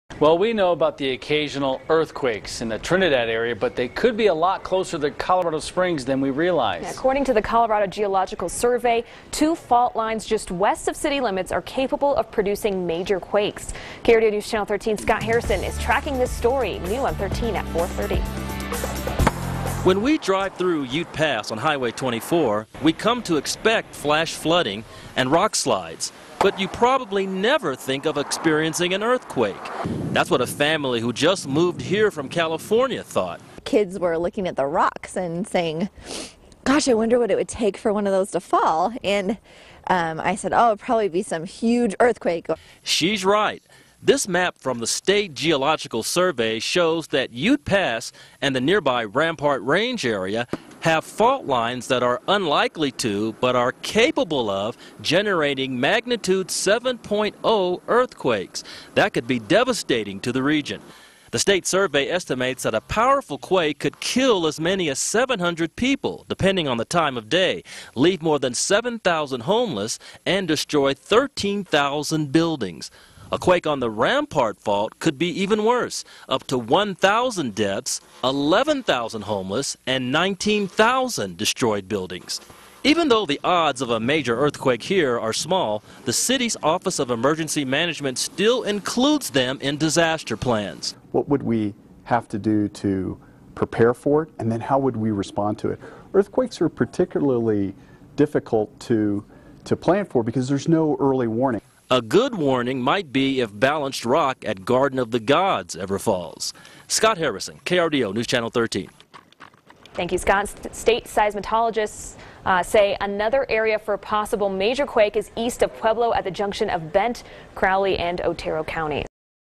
Earthquake Fault Lines Lie Close to Colorado Springs
State seismologists update damage estimates for major temblor.